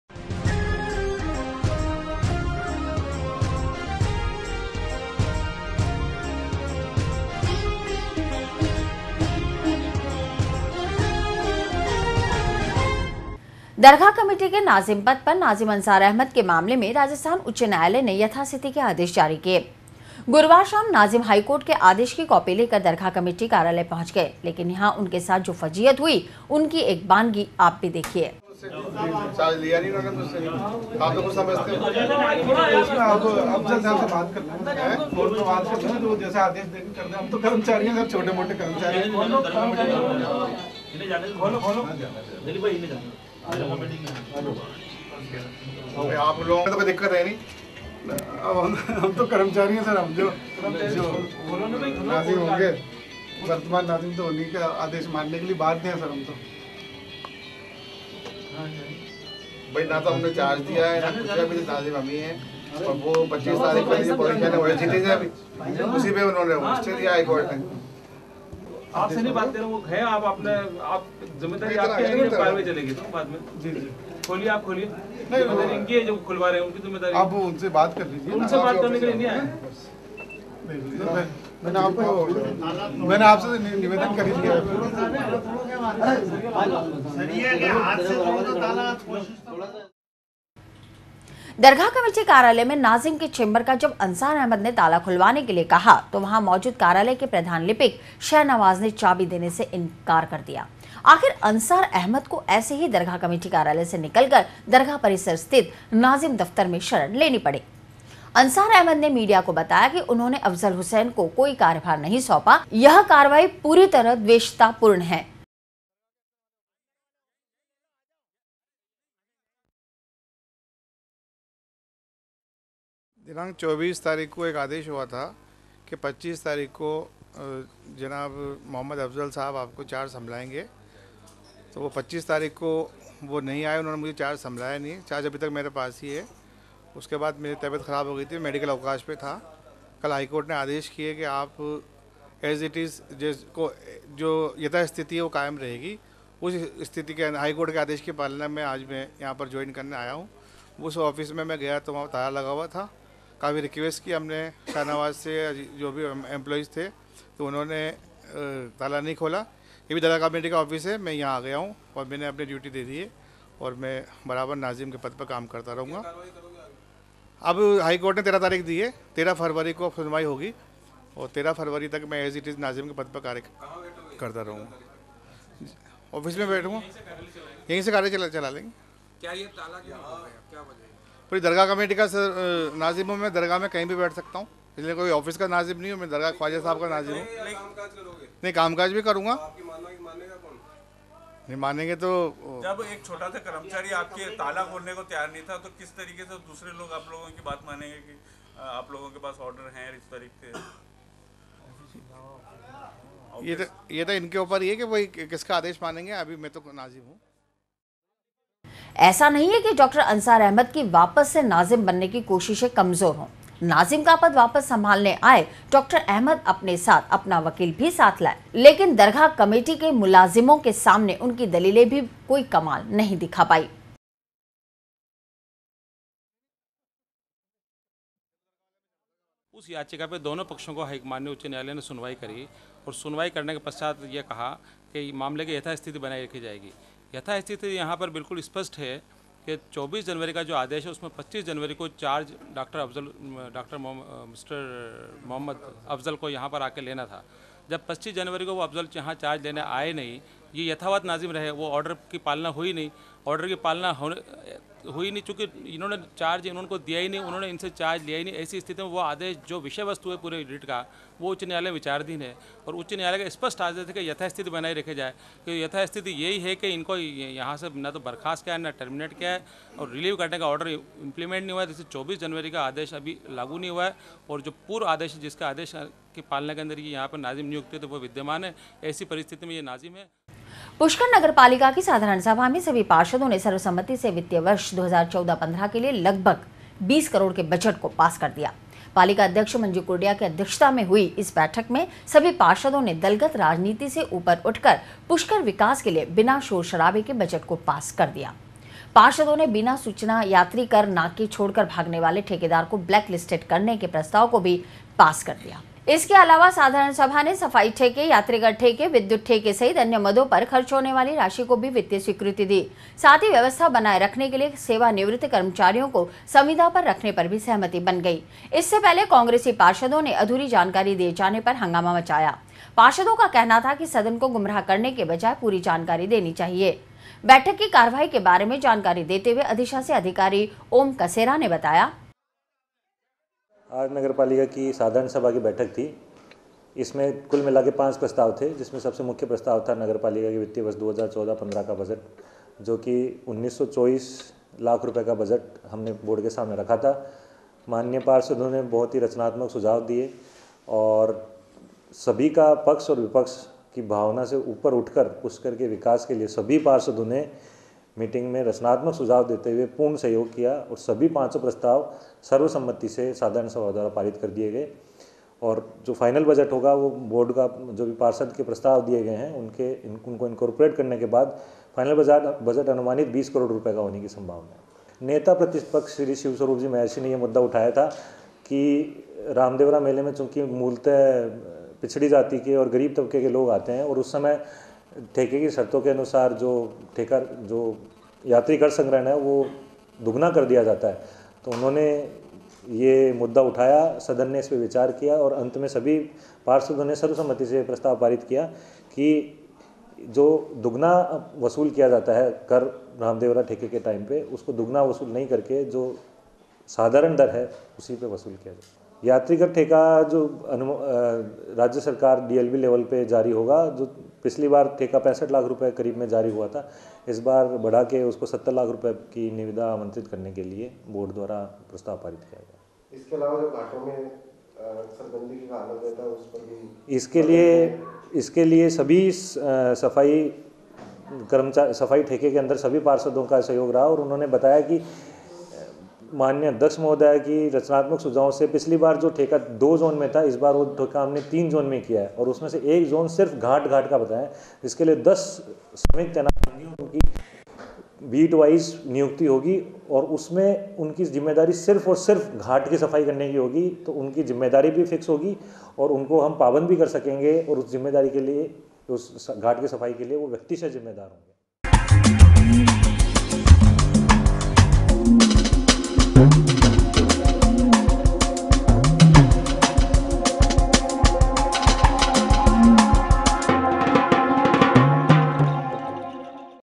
दरगाह नाजिम का विवाद नही ले रहा थमने का नाम।
दरगाह नाजिम का विवाद नही ले रहा थमने का नाम। हाईकोर्ट का आदेश लेकर वकील के साथ पहुंचे अंसार अहमद। कार्यालय का कर्मचारीयों ने नही खोला ताला।